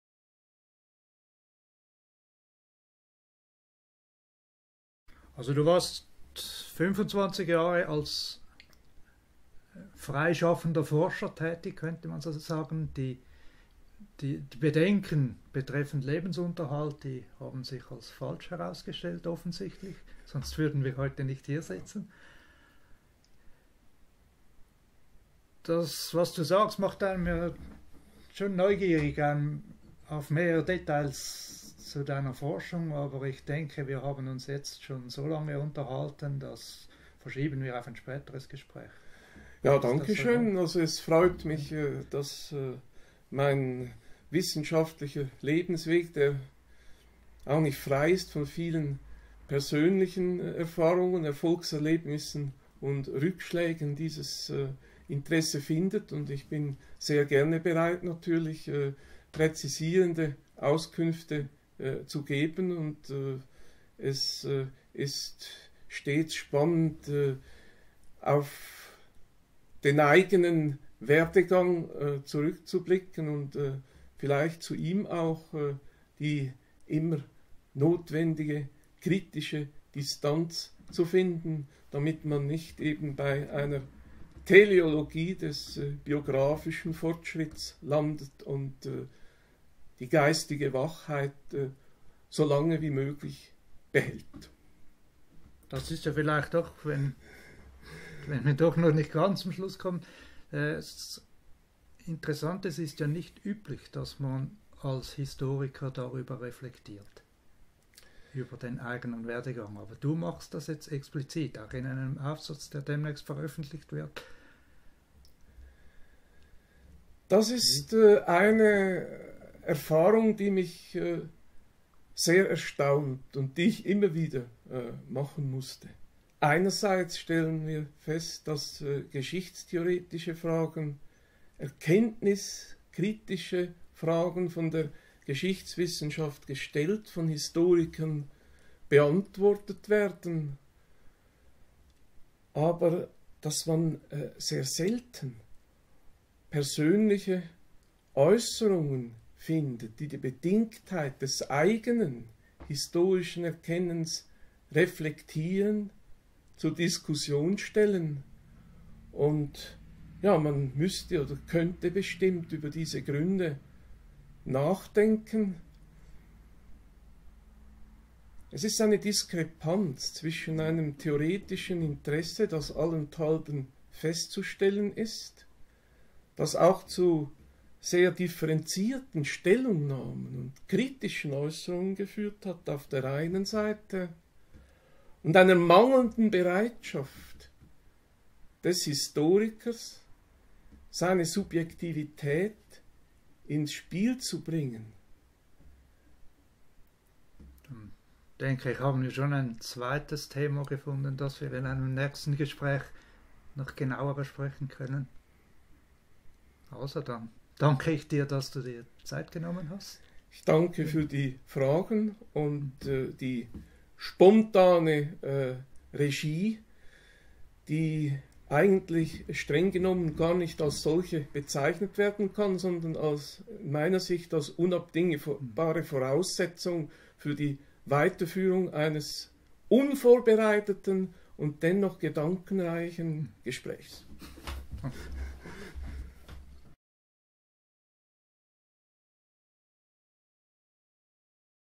Dr. Hanspeter Marti 10/10: Kritische Distanz zum eigenen Werdegang
Biographisches Interview mit Dr. Hanspeter Marti, Arbeitsstelle für kulturwissenschaftliche Forschungen, Engi, Schweiz
Teil 10: Kritische Distanz zum eigenen Werdegang